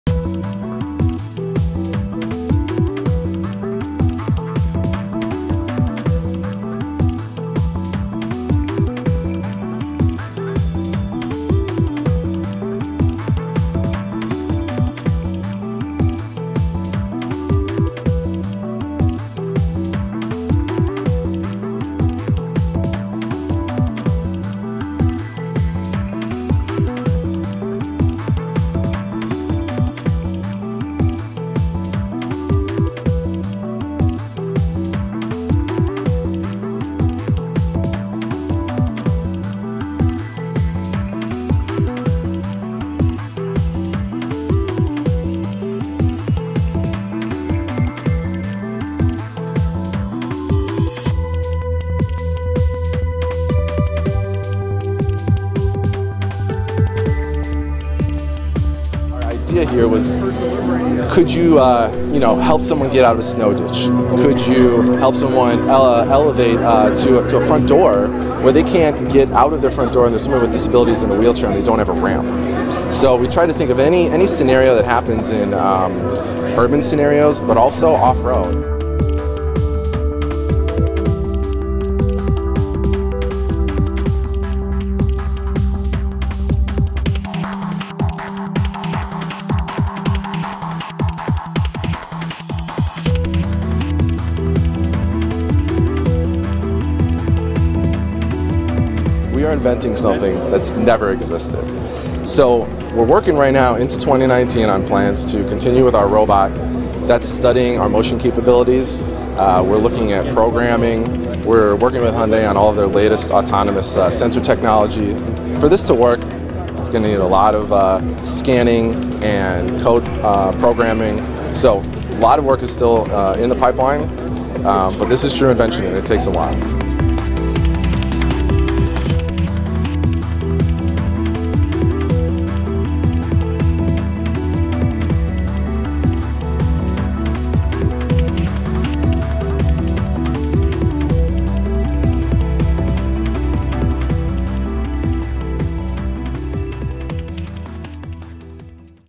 Este prototipo de vehículo terrestre se puede mover como un mamífero o un reptil
El diseñador jefe avisa: "Esto es invención pura. Estamos inventando algo que nunca existió”.